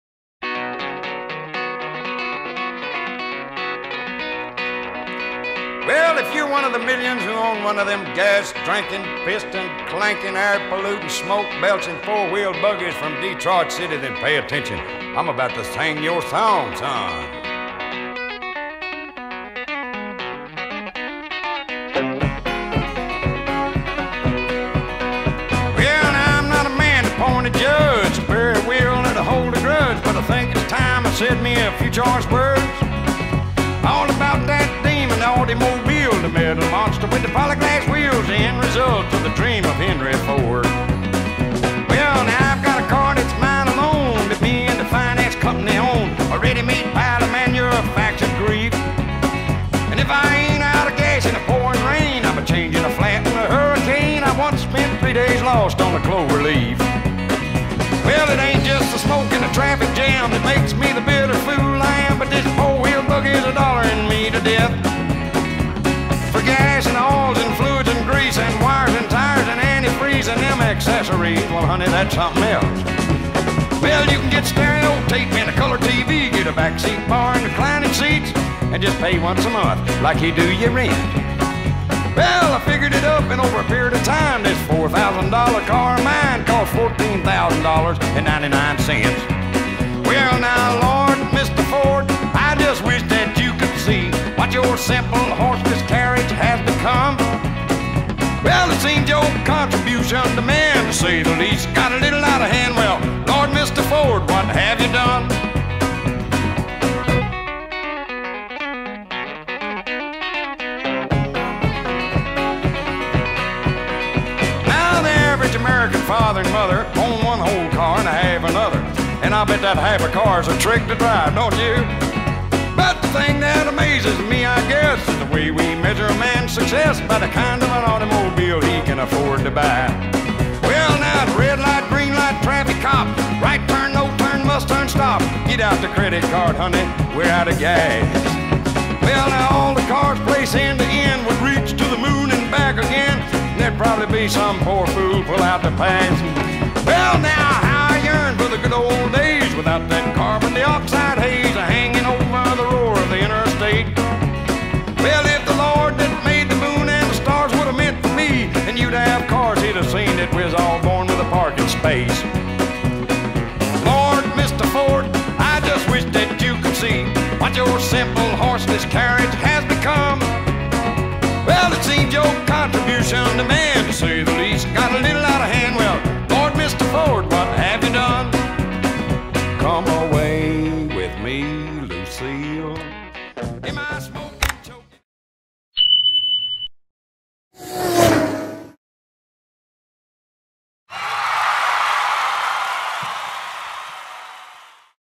U.S. Racetrack Tour 2024 - Thunderhill
Barrel a monkeys U.S. Racetrack tour 2024 first stop, Thunderhill.  Spec Racer Ford